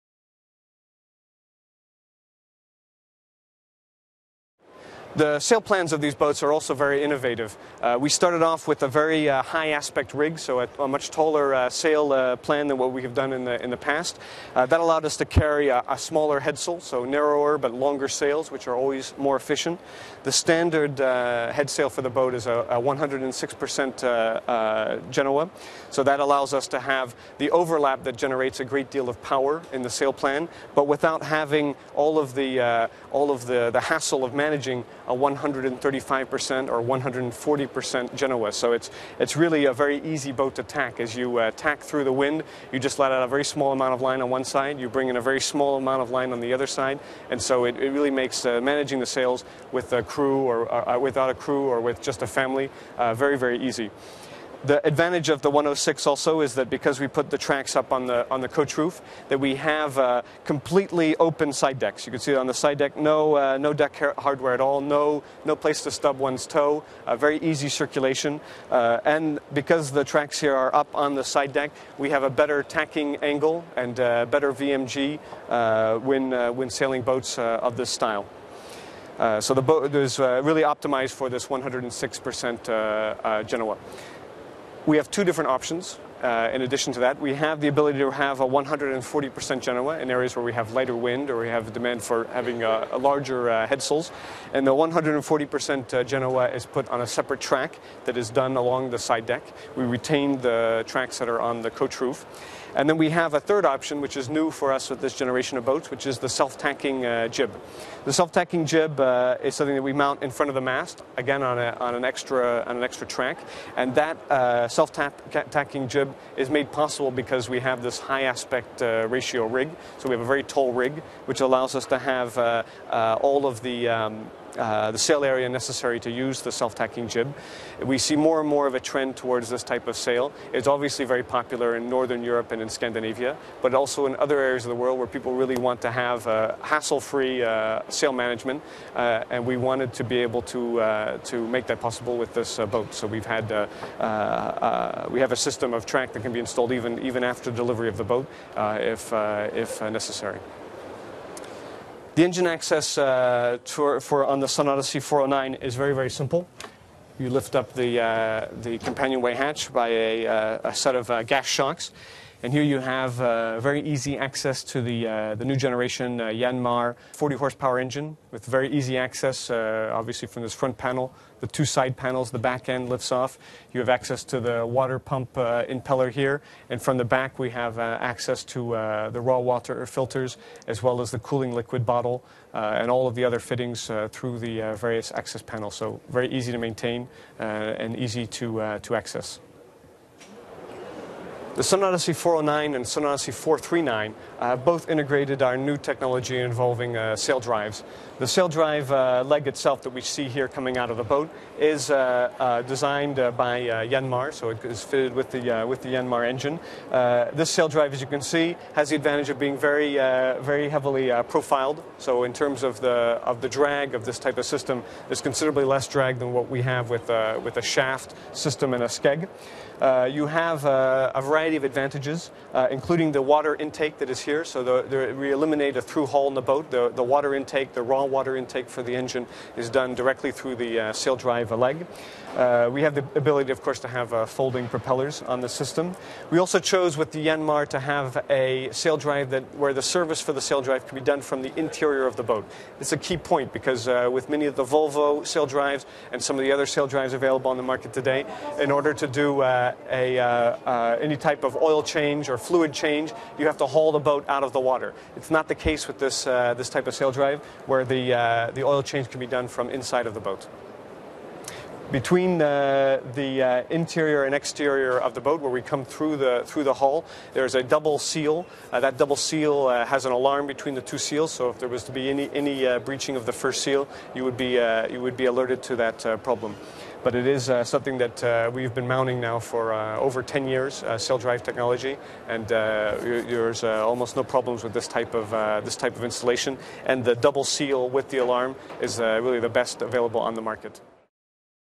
Jeanneau Sun Odyssey 409 Propulsion(Archive Boat Sales Promotion Video)
The Jeanneau Sun Odyssey 409 has already received 2010/2011 European Yacht of the Year Award. This nomination comes from the major European magazines from 11 different countries.